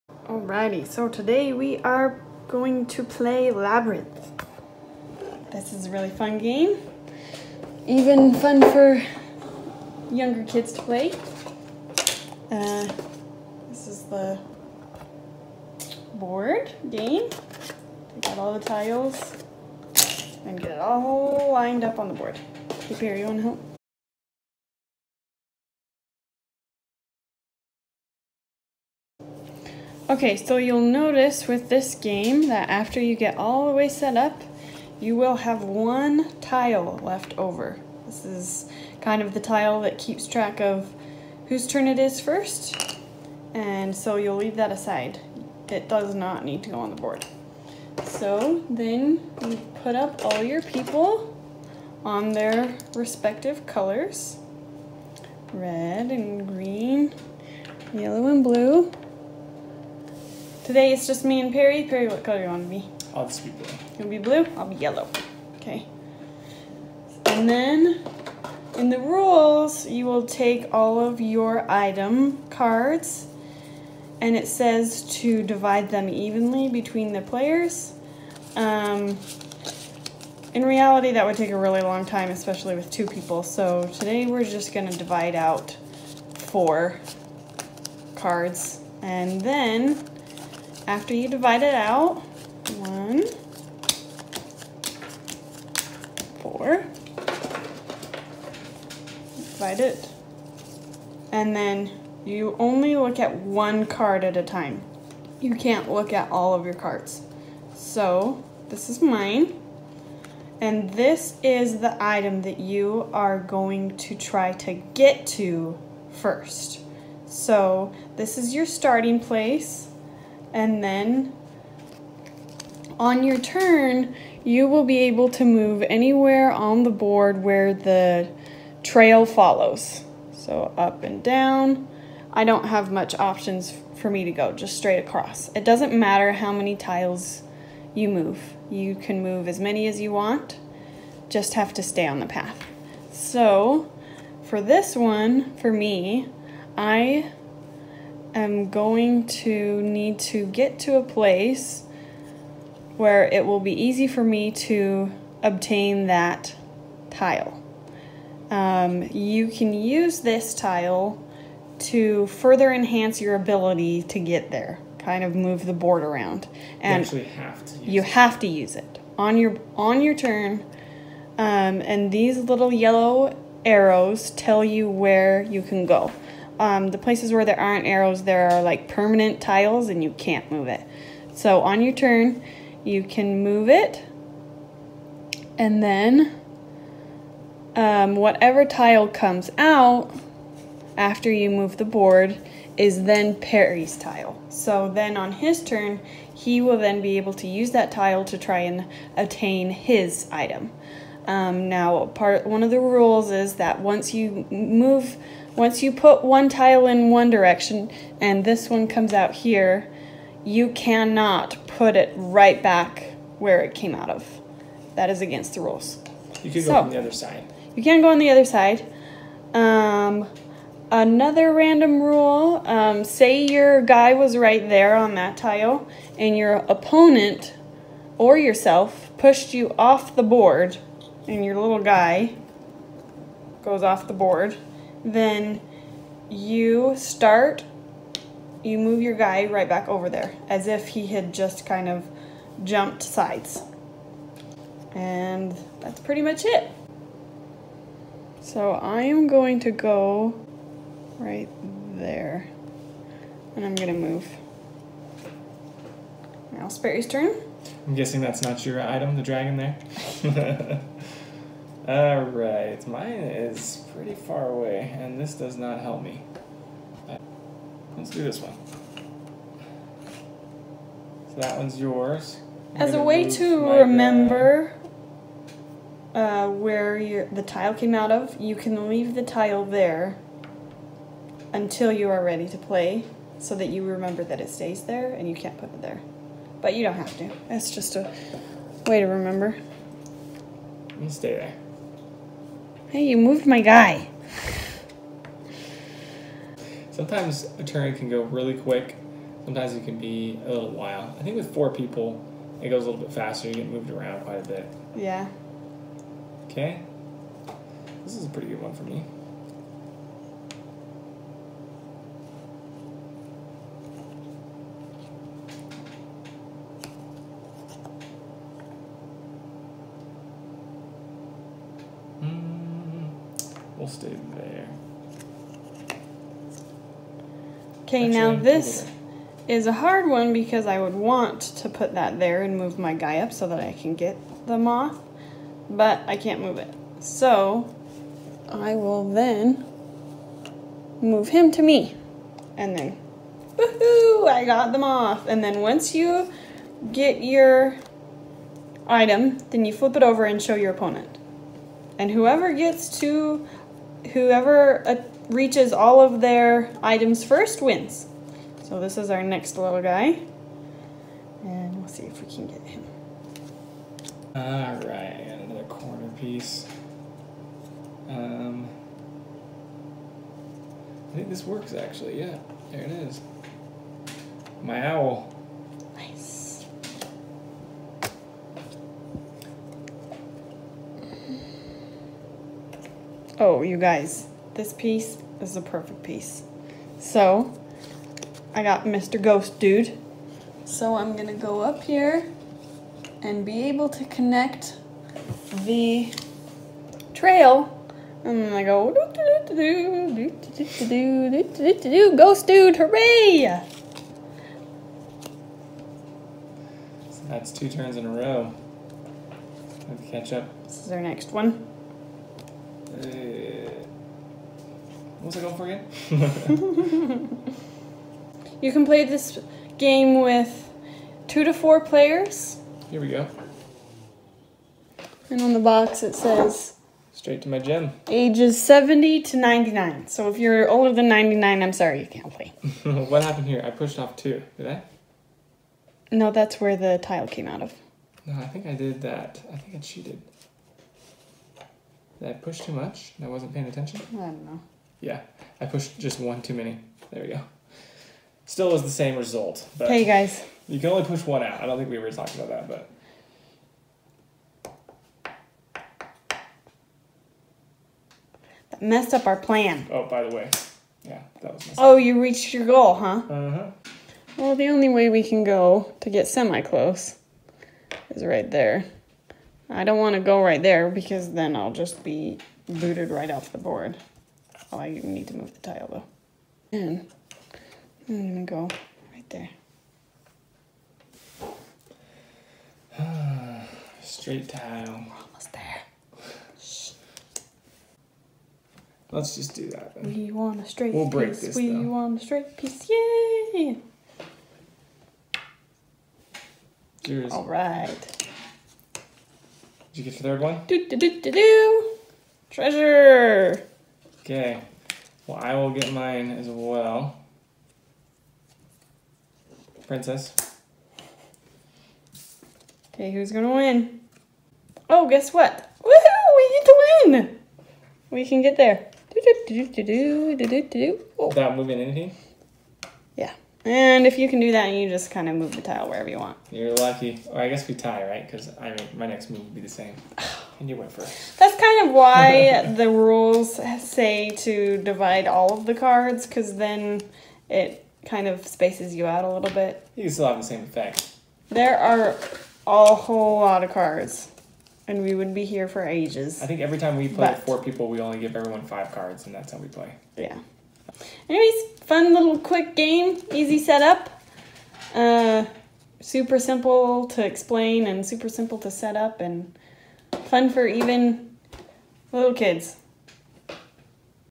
how to play the board game labyrinth
This board game is very simple to pick up and teach to new players. It can be a fun race between two players or a good competition between 3 to 4 players. Gameplay can also be pretty quick when played with a few veterans of the game. We would definitely classify it as a puzzle game but there are some elements of strategy since your opponents will be effecting how you move and reach your treasure.